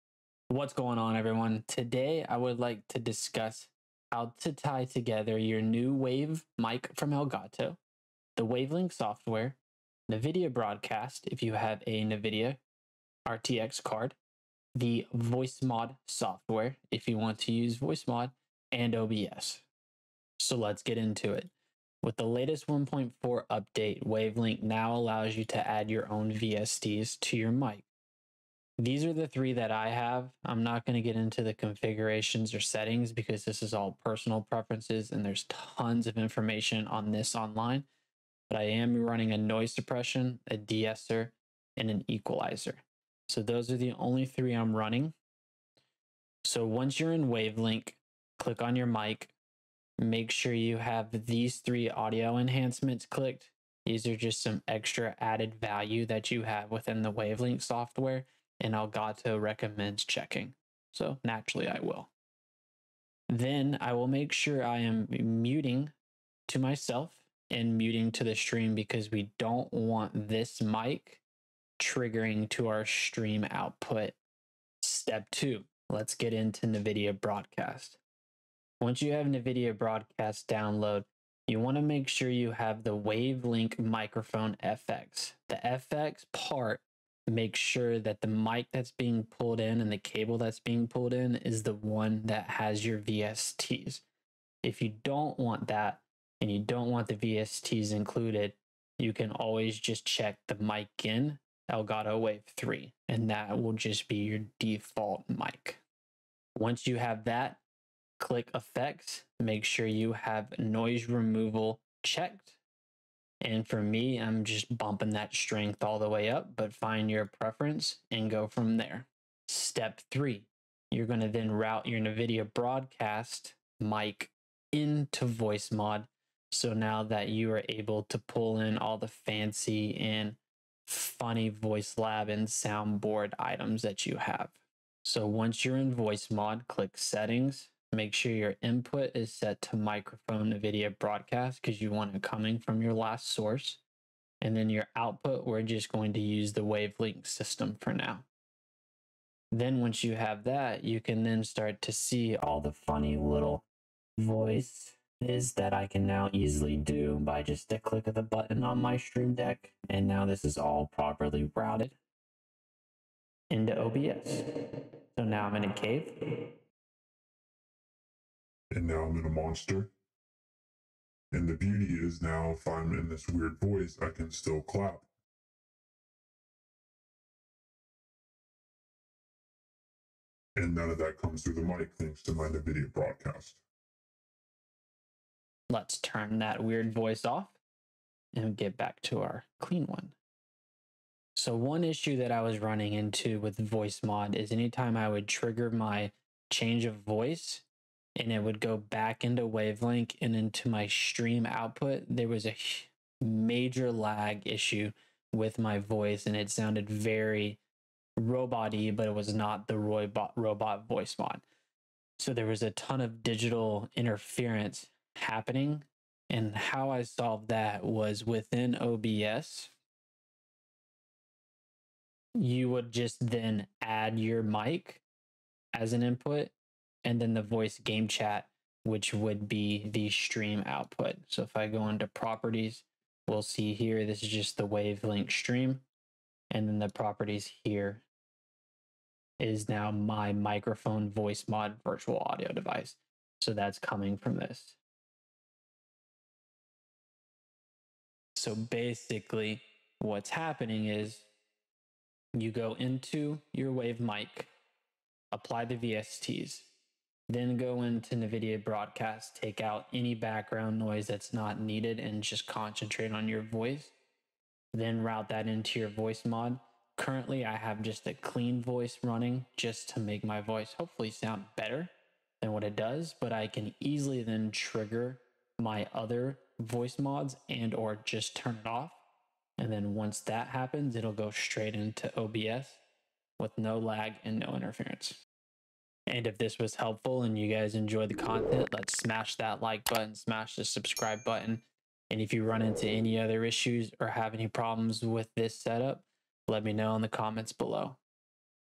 HOW TO CONNECT ELGATO WAVE LINK TO NVIDIA BROADCAST TO VOICEMOD TO OBS
Lets learn how to connect your elgato wave mic or xlr with wave link software to nvidia broadcast then to voicemod then back into OBS with now audio clipping or issues.

🔴 Watch Me Live Now on Youtube 🔴
🔥Social Medias 🔥

🕘Timestamps:

00:00 Intro
00:00:22 Step 1 - Setting up your wave link
00:01:14 Step 2 - Setting up your nvidia broadcast
00:01:59 Step 3 - Setting up your voicemod
00:03:37 Step 4 - Setting up your OBS
00:04:10 Recap